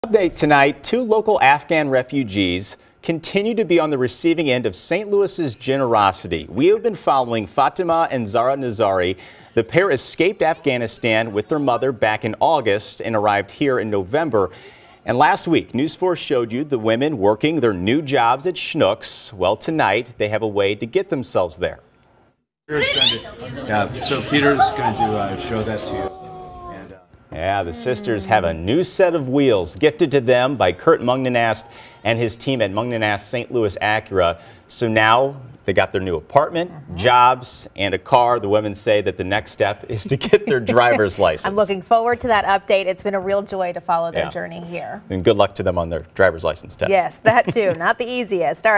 St. Louis dealership donates car to Afghan sisters, mother who escaped Taliban
A St. Louis car dealership is helping a family of Afghan refugees who fled their home country last August